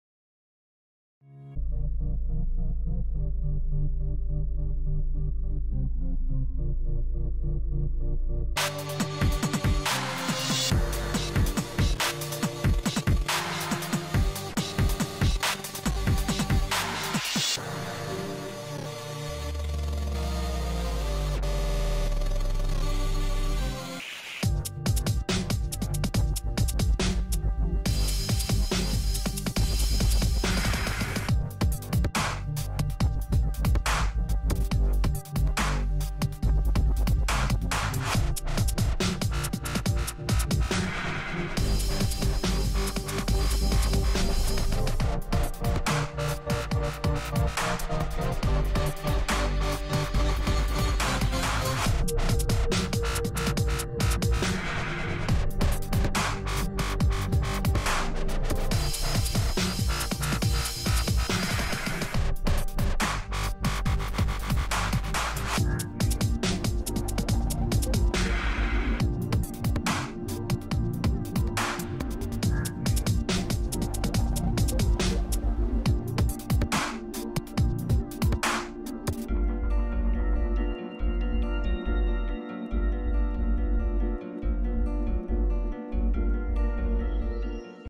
MWC 3D Rendering
This video shows the 3d rendering of the mobile world congress